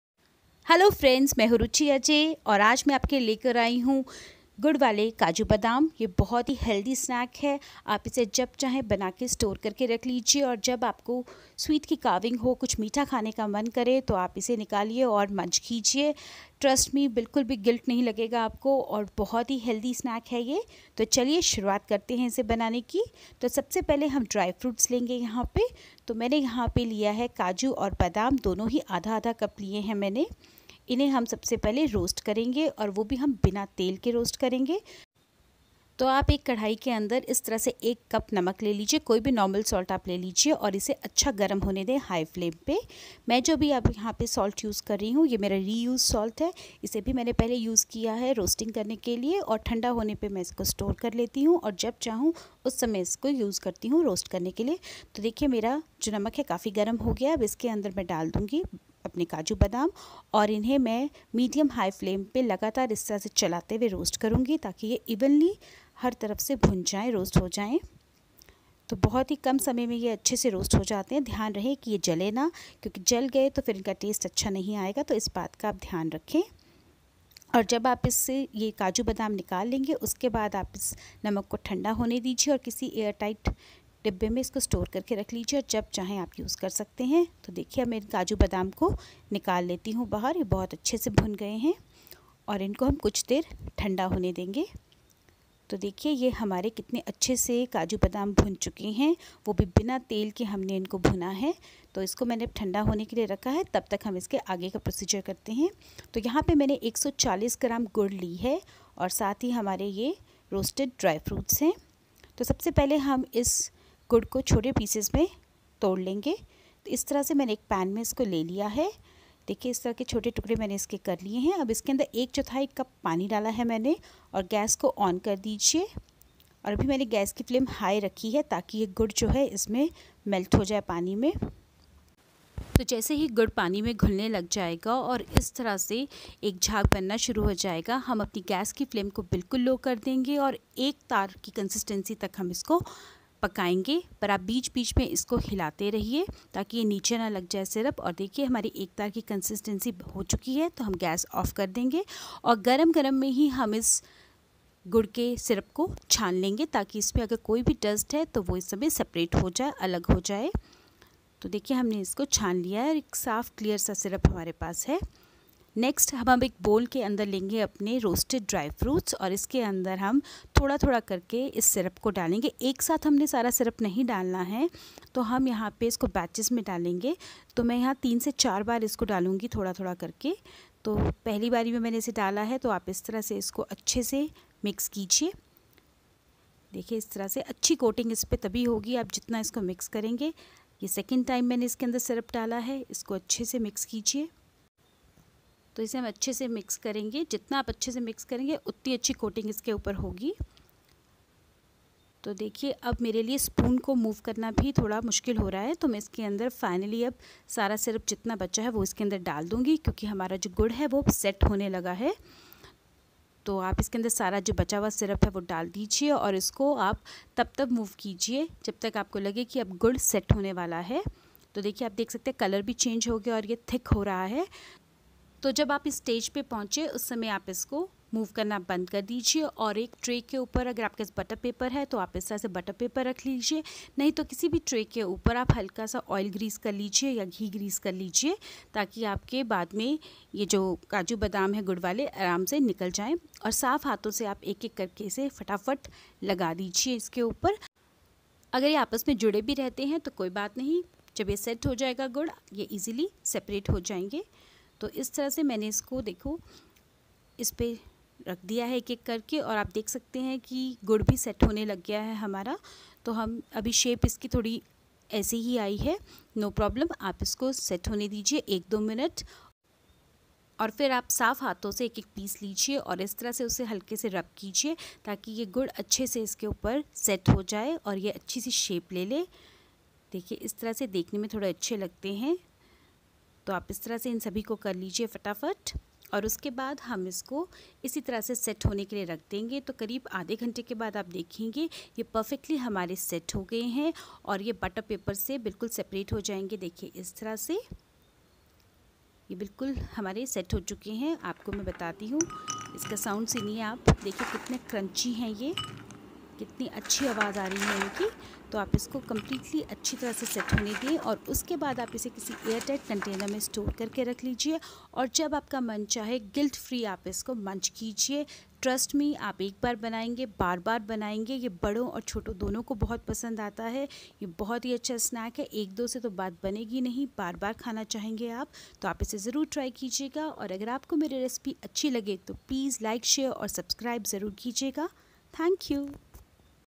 Healthy Snack/Roasted dry fruits/ Dry fruits coated with jaggery/गुड वाले काजू-बादाम
In this video i have shared recipe of how to roast dry fruits without oil amd coat them with jaggery.Healthy and yummy snack recipe...Do try this recipe